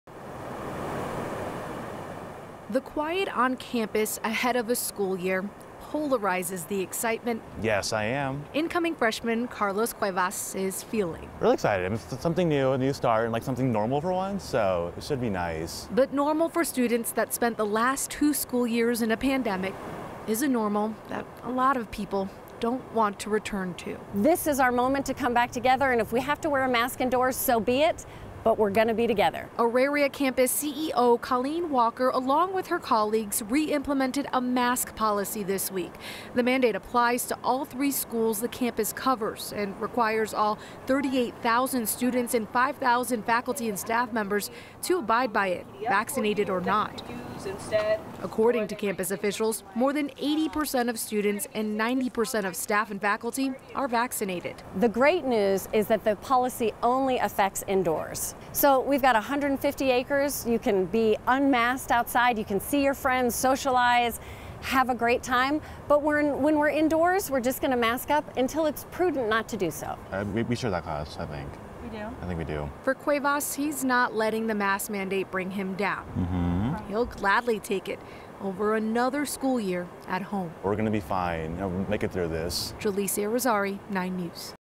Auraria campus requiring indoor mask-wearing, vaccinated or not
According to Auraria campus officials more than 80% of students and 90% of staff and faculty are vaccinated.

9NEWS (KUSA) is located in Denver, Colorado